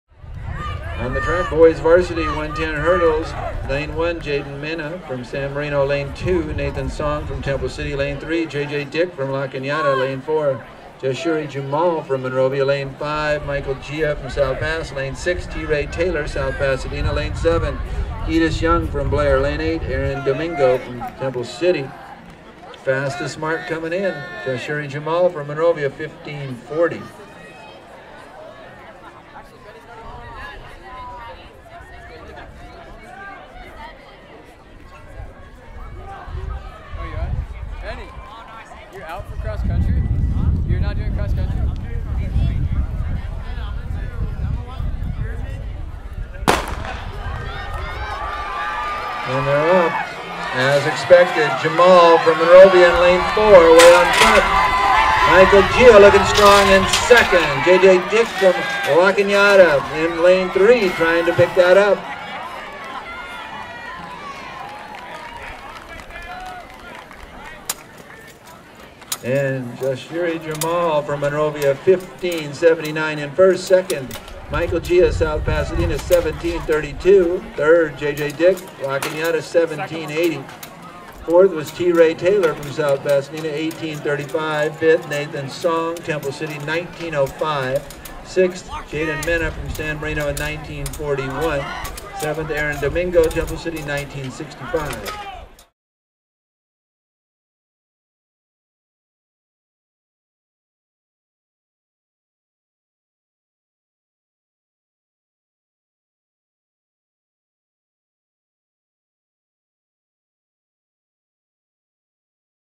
2019 TF - Rio Hondo Finals - 110 Meter Hurdles (Varsity Boys)
Here is the Varsity Boys 110 Meter Hurdles from the 2019 Rio Hondo League Track & Field Finals Meet, held at South Pasadena High School (California) on April 26, 2019.  The meet determines both Rio Hondo League rankings as well as which Varsity athletes will proceed to CIF competition.

Top 5
1       Jasiri Jumal        12 Monrovia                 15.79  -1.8
2       Michael Jia         11 South Pasadena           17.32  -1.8
3       JJ Dick             11 La Canada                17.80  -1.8
4       Teray Taylor        11 South Pasadena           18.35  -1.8
5       Nathan Song         12 Temple City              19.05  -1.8

An indexed and YouTube-linked list of all of our videos from Rio Hondo Finals will (eventually) be posted to: RioHondoLeague.com